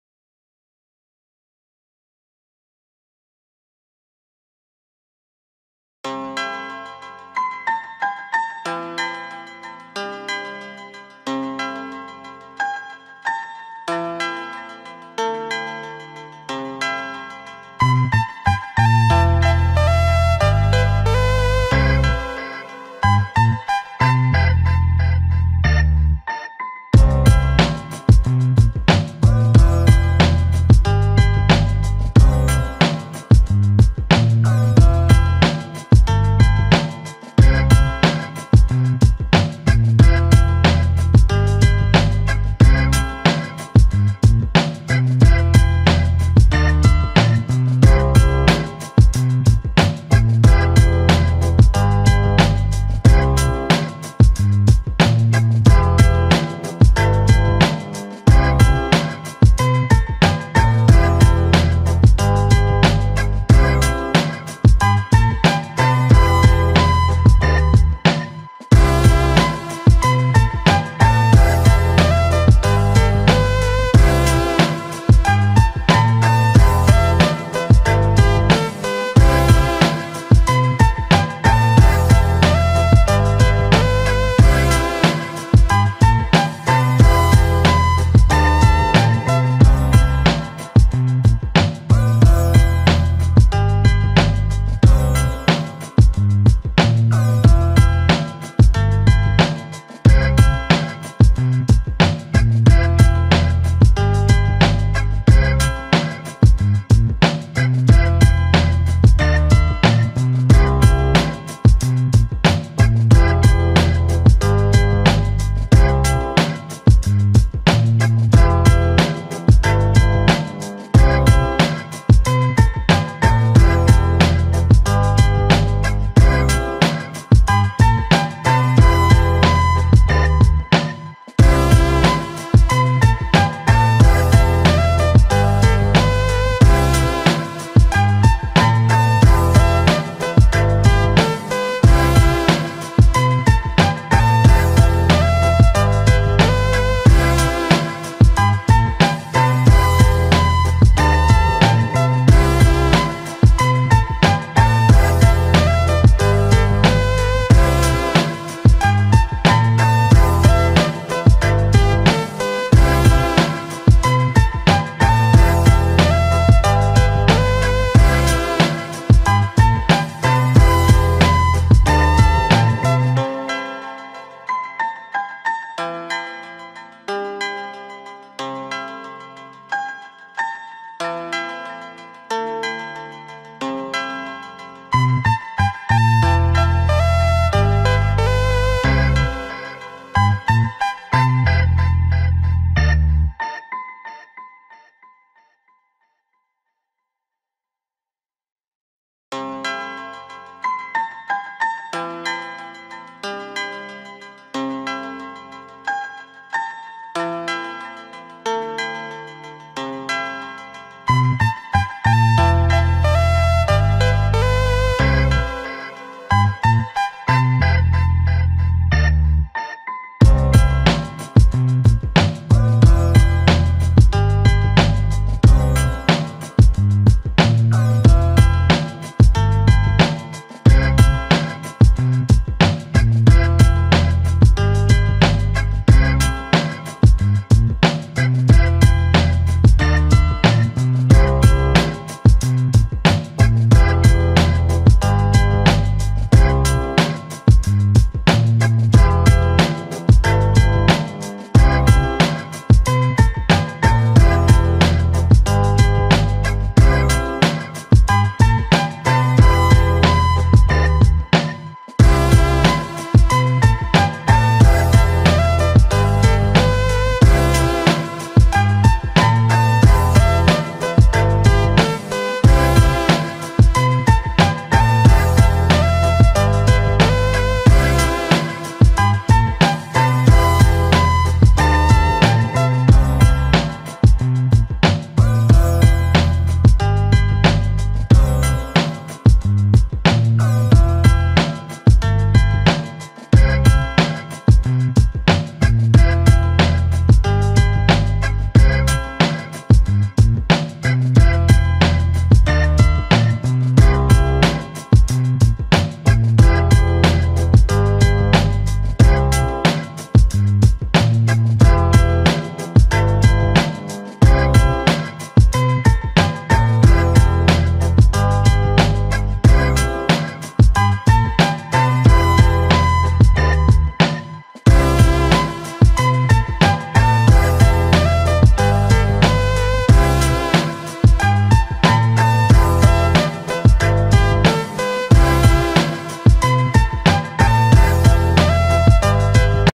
How to Draw a Realistic Butterfly with Colored Pencils: Time Lapse |JOY IN THE BOX
Easy way to draw realistic butterfly : time laps drawing.
ART MATERIAL
1. cartridge paper
2. prisma color pencil
music credit

happy drawing
joy
happydrawing95i